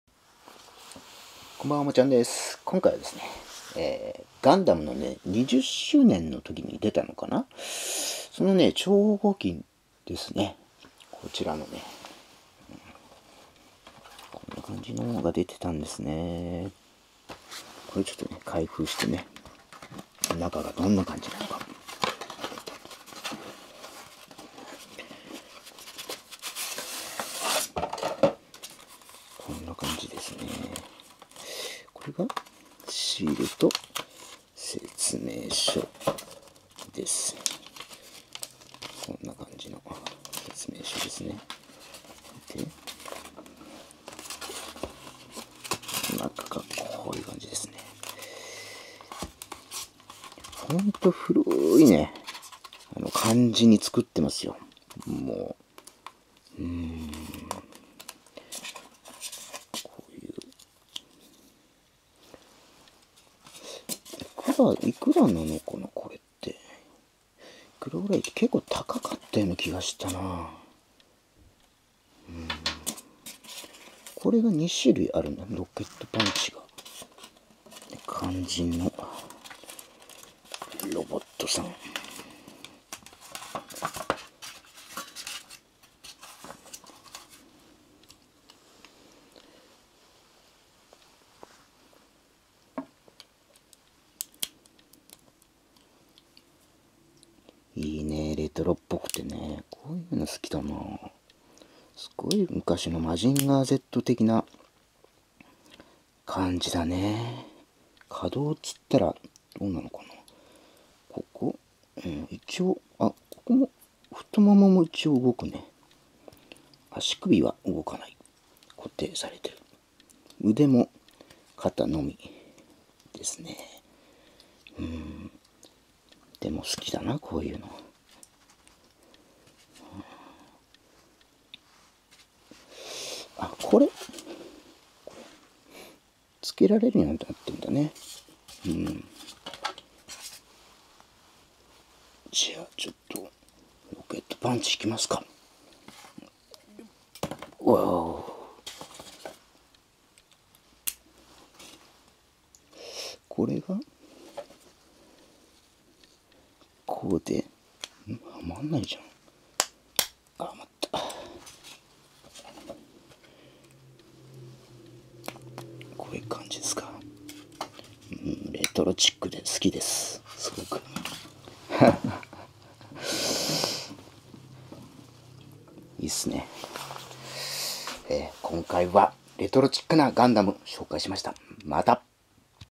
レトロな超合金ガンダム
２０周年で販売されたレトロな超合金ガンダムです。マジンガーZ世代は懐かしい感じがします。